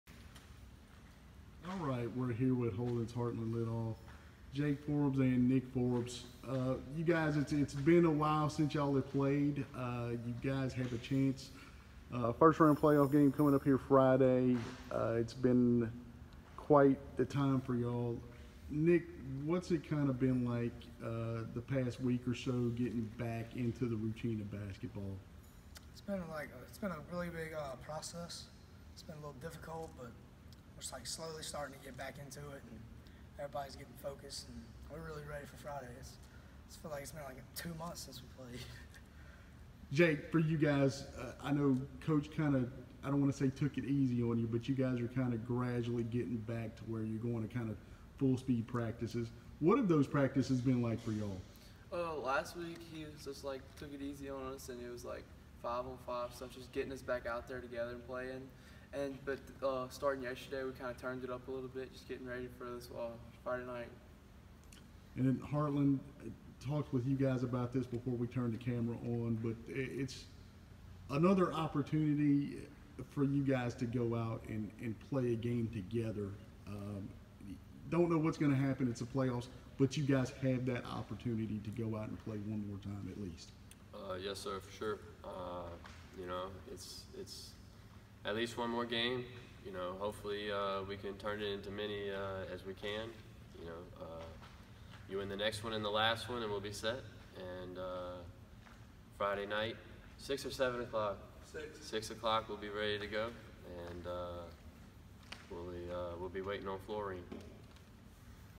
Holden boys to host Florien in Class B playoff opener
Holden's Hartland Litolff, Nick Forbes and Jake Forbes discuss getting back to basketball.